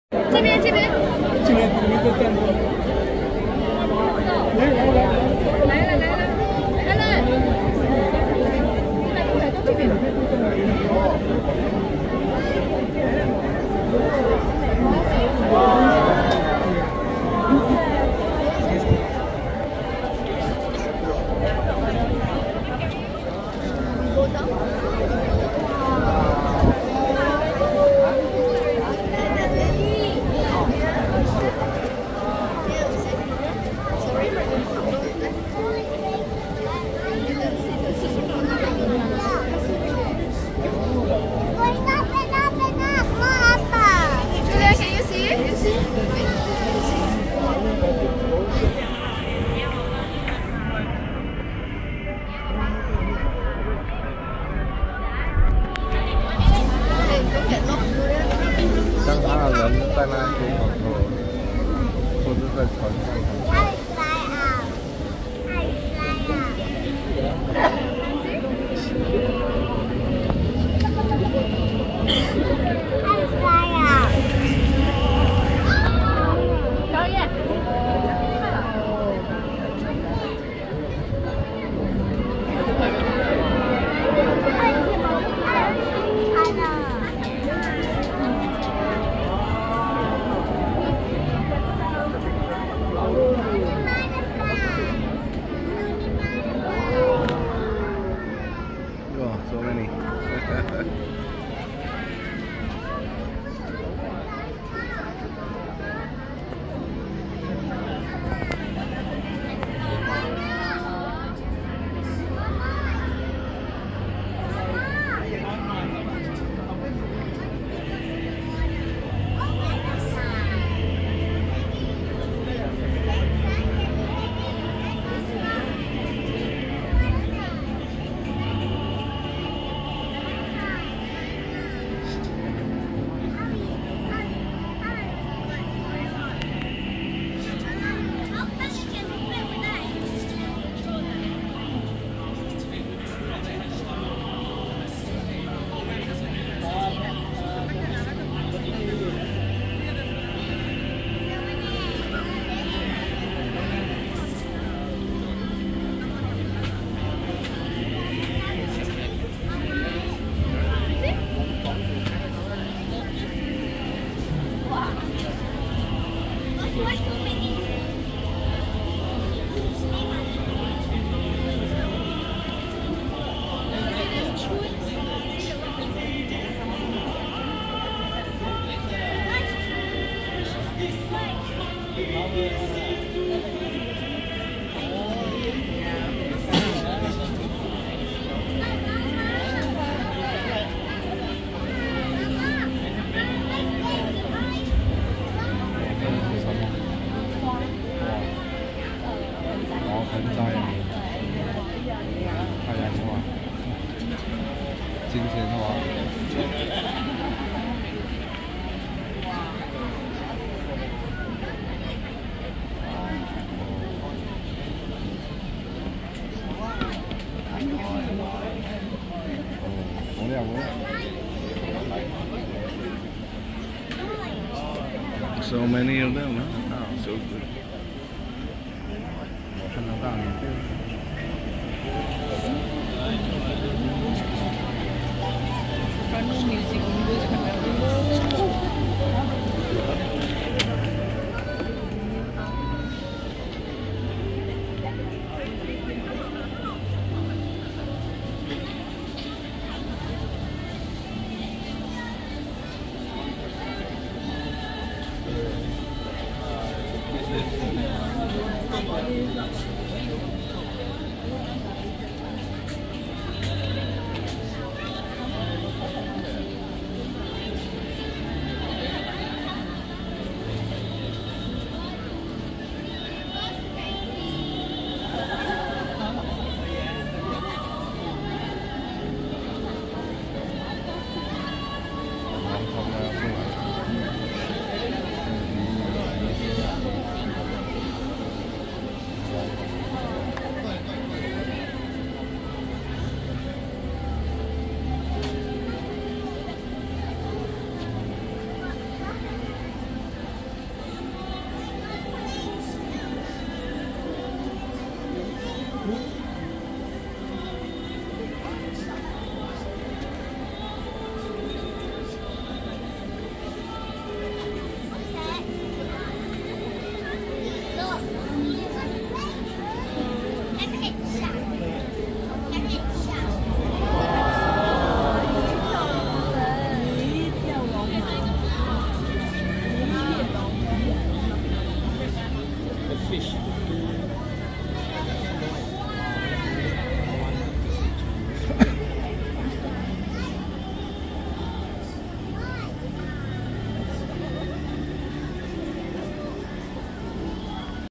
MBS Lunar New Year of the Dragon 2024 Drones Light Show 01
Marina Bay Sands Lunar New Year of the Dragon Drones Light Show